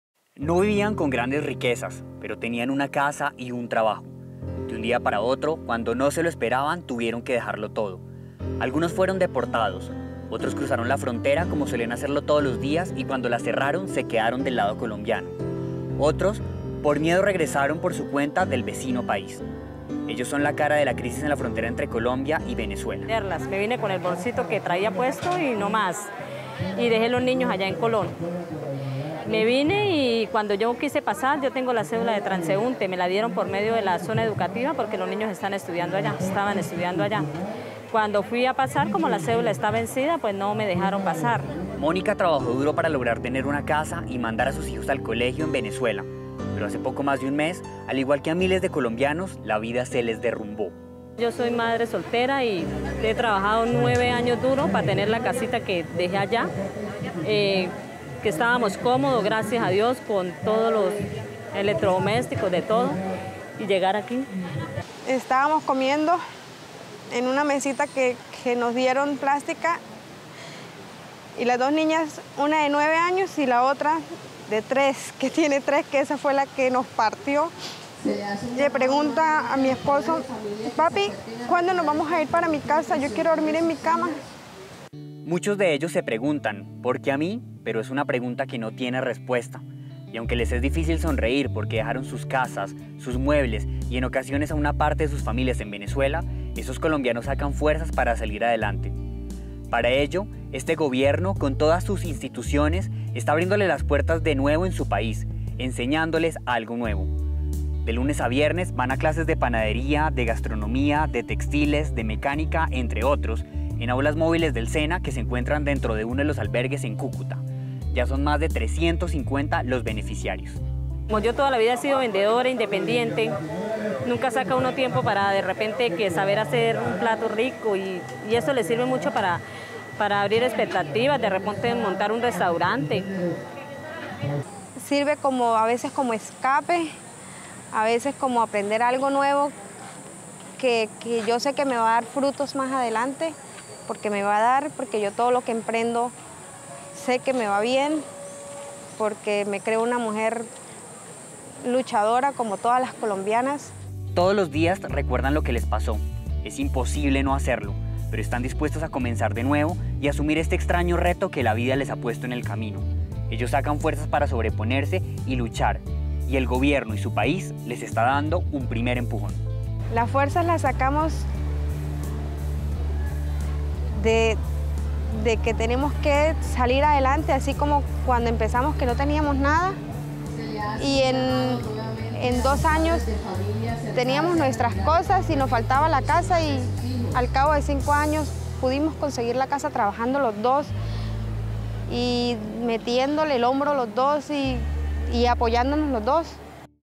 Beneficiarios aulas móviles del SENA en Cúcuta
Algunas de las personas que fueron deportadas de Venezuela, van de lunes a viernes a clases de panadería, de gastronomía, de textiles y de mecánica, entre otros, en aulas móviles del Sena, que se encuentran dentro de uno de los albergues en Cúcuta. ¡Ya son más de 350 beneficiarios!